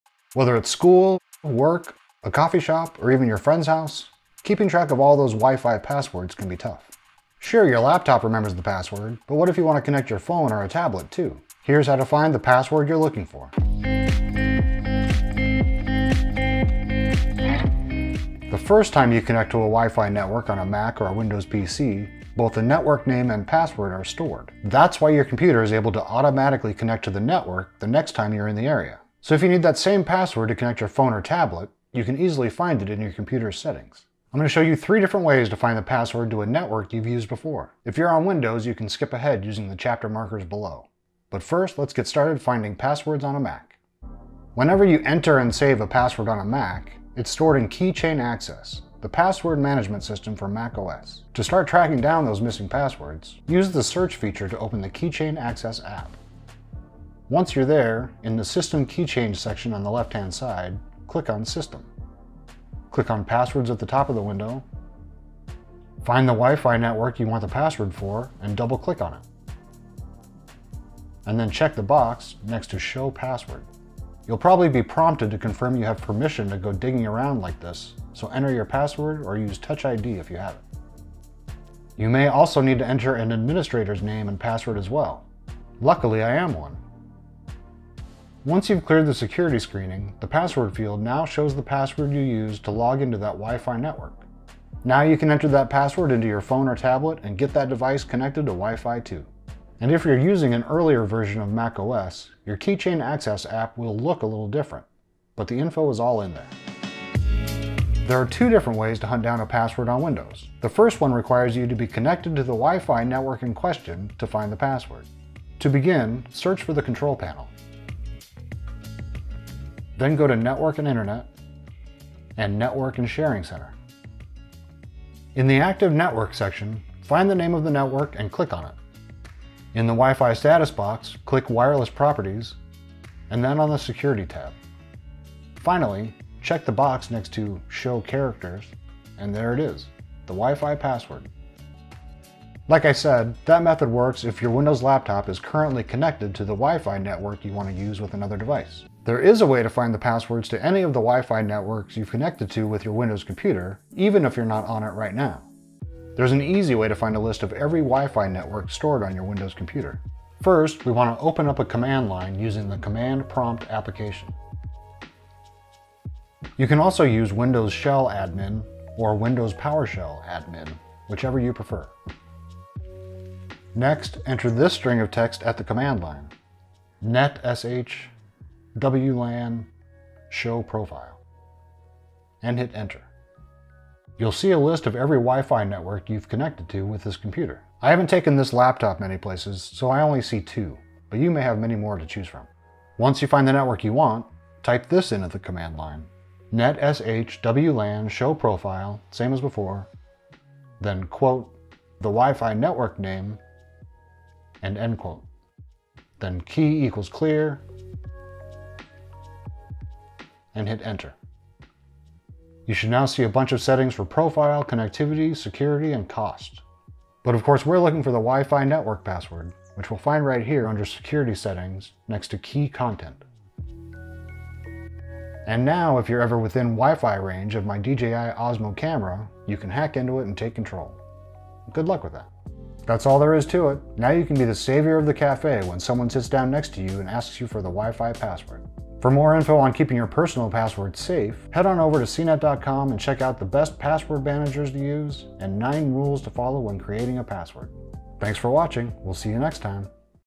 How to Find the Password for Any Wi-Fi Network You've Connected To
It’s easy to forget the Wi-Fi passwords of places you frequent -- like coffee shops, libraries, school and work -- but we can help you find them. Your computer has likely saved the Wi-Fi passwords for any networks you've signed into.

0:00 Find Forgotten Wi-Fi Passwords
0:44 Finding Wi-Fi Passwords on MacOS
1:47 Finding Wi-Fi Passwords on Windows (Option 1)
2:35 Finding Wi-Fi Passwords on Windows (Option 2)